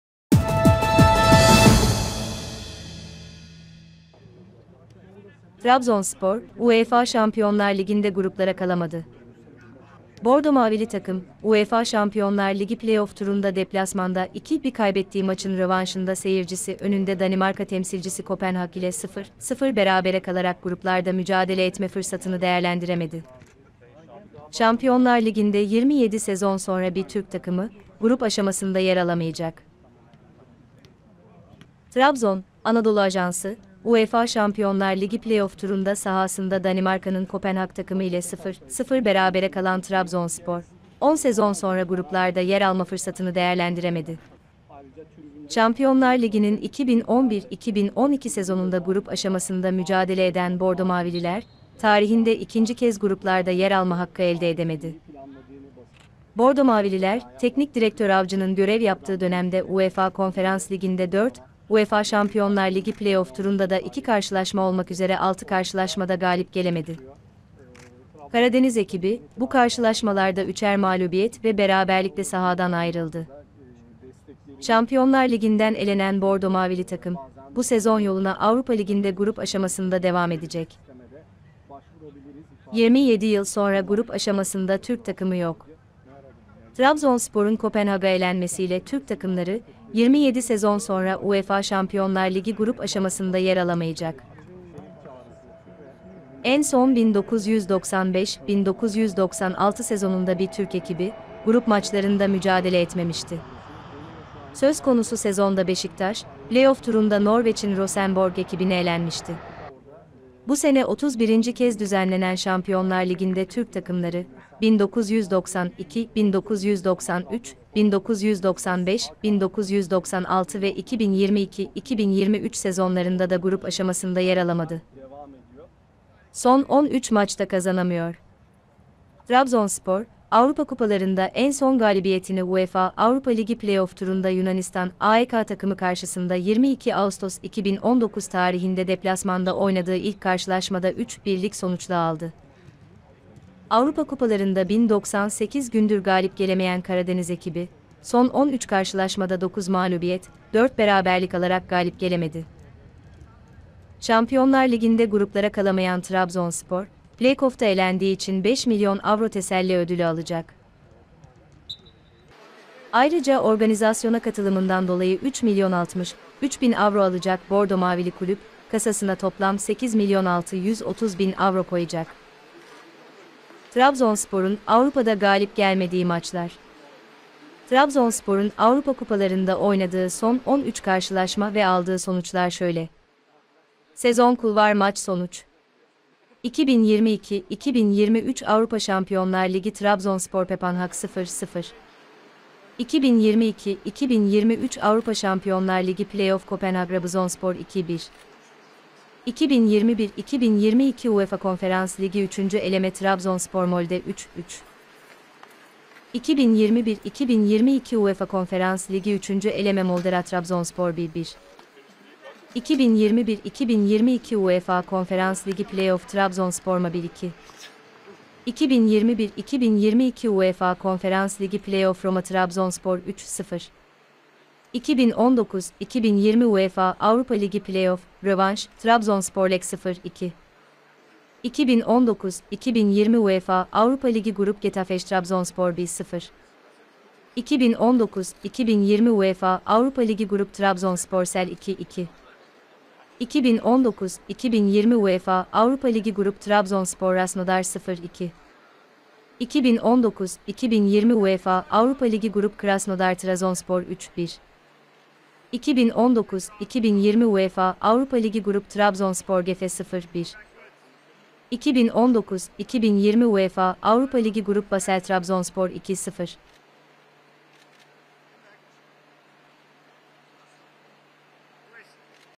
Trabzonspor, UEFA Şampiyonlar Ligi'nde Gruplara Kalamadı
UEFA Şampiyonlar Ligi play-off turunda sahasında Danimarka'nın Kopenhag takımı ile 0-0 berabere kalan Trabzonspor, 10 sezon sonra gruplarda yer alma fırsatını değerlendiremedi.
Şampiyonlar Ligi'nin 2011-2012 sezonunda grup aşamasında mücadele eden bordo-mavililer, tarihinde ikinci kez gruplarda yer alma hakkı elde edemedi.
Bordo-mavililer, teknik direktör Avcı'nın görev yaptığı dönemde UEFA Konferans Ligi'nde 4, UEFA Şampiyonlar Ligi play-off turunda da 2 karşılaşma olmak üzere 6 karşılaşmada galip gelemedi.
Karadeniz ekibi, bu karşılaşmalarda üçer mağlubiyet ve beraberlikle sahadan ayrıldı.
Şampiyonlar Ligi'nden elenen bordo-mavili takım, bu sezon yoluna Avrupa Ligi'nde grup aşamasında devam edecek.